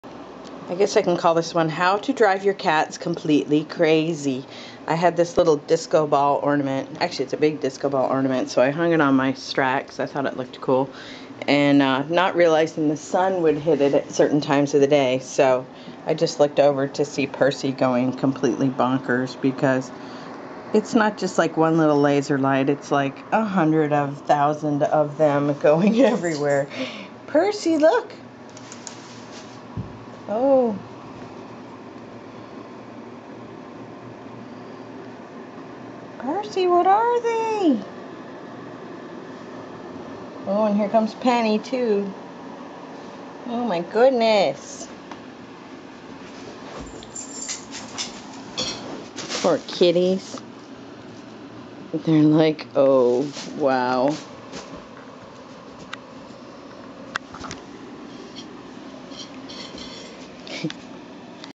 Too many dots!!!!
Who needs a laser pointer when you have a disco ball and sunlight?